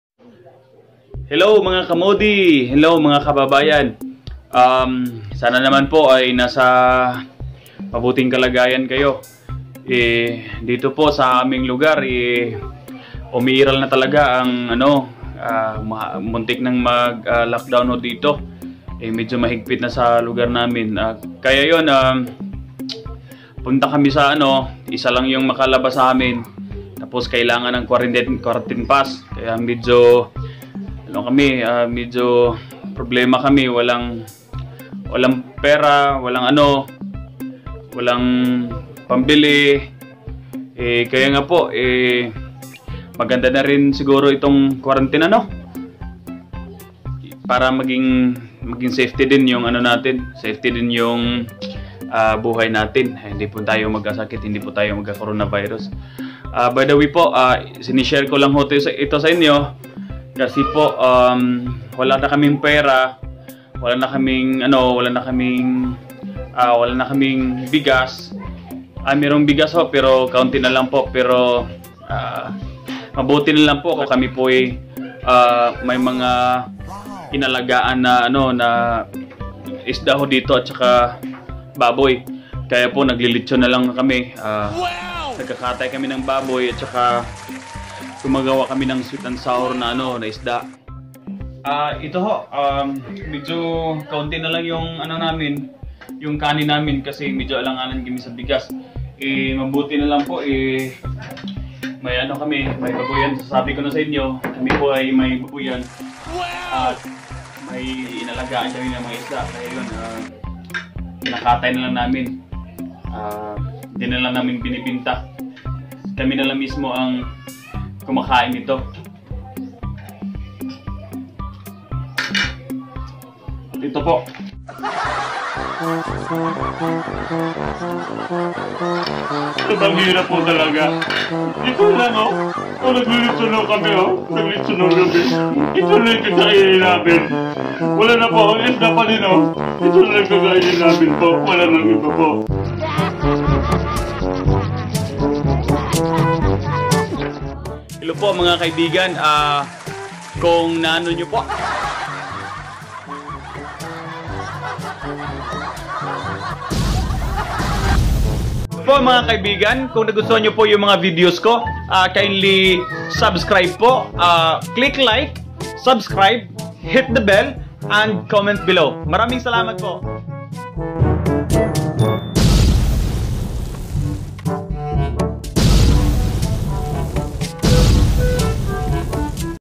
Tatak comedy#3..buti na lang may piggery eyy Kung wala, eyy wala talagang makain..Mr. President!!!
Naglechon na Lang kami..d kase makalabas dahil enhancing community quarantine kase..